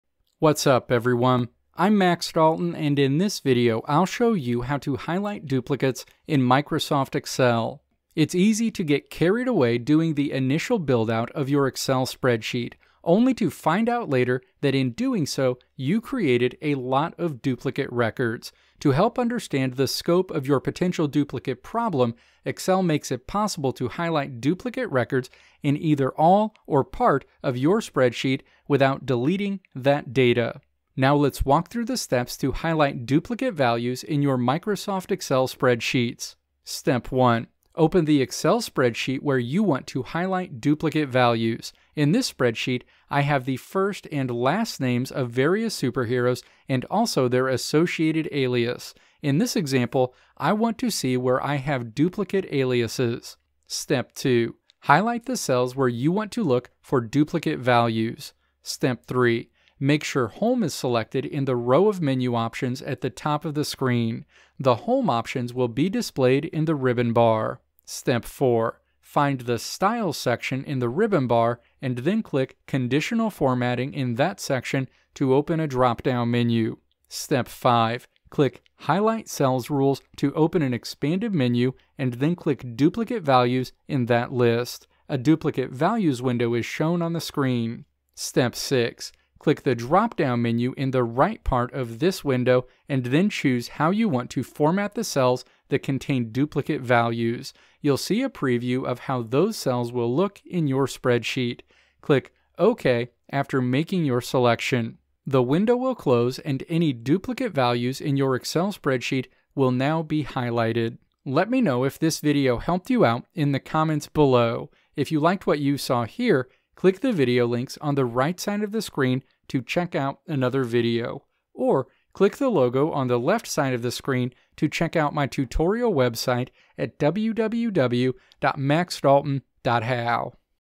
How to Highlight Duplicates in Microsoft Excel
In this video I'll show you how to highlight duplicates in Microsoft Excel.

Timestamps:
Introduction: 0:00
Steps to Highlight Duplicates in Microsoft Excel: 0:30
Conclusion: 1:51

Video Transcript:
It's easy to get carried away doing the initial buildout of your Excel spreadsheet, only to find out later that in doing so you created a lot of duplicate records. To help understand the scope of your potential duplicate problem, Excel makes it possible to highlight duplicate records in either all or part of your spreadsheet without deleting that data.

Now let's walk through the steps to highlight duplicate values in your Microsoft Excel spreadsheets.

Step 1. Open the Excel spreadsheet where you want to highlight duplicate values. In this spreadsheet I have the first and last names of various superheroes, and also their associated alias. In this example, I want to see where I have duplicate aliases.

Step 2. Highlight the cells where you want to look for duplicate values.

Step 3. Make sure "Home" is selected in the row of menu options at the top of the screen. The Home options will be displayed in the Ribbon bar.

Step 4. Find the Styles section in the Ribbon bar, and then click "Conditional Formatting" in that section to open a drop-down menu.

Step 5. Click "Highlight Cells Rules" to open an expanded menu, and then click "Duplicate Values" in that list. A Duplicate Values window is shown on the screen.

Step 6. Click the drop-down menu in the right part of this window, and then choose how you want to format the cells that contain duplicate values. You'll see a preview of how those cells will look in your spreadsheet. Click "OK" after making your selection. The window will close and any duplicate values in your Excel spreadsheet will now be highlighted.